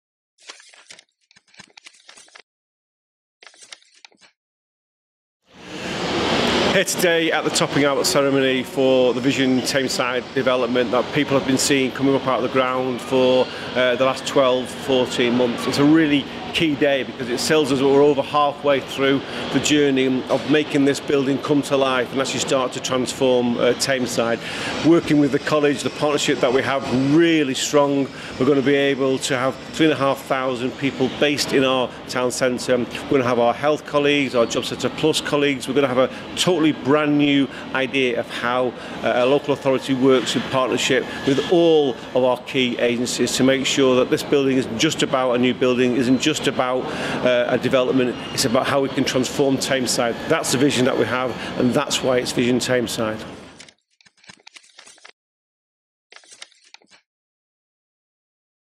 Leader's Vlog: Joint Service Centre Topping Out
The Executive Leader of the Council, Cllr Kieran Quinn, talks about the progress made on the new Joint Service Centre and what it means for Tameside, the council and its partners.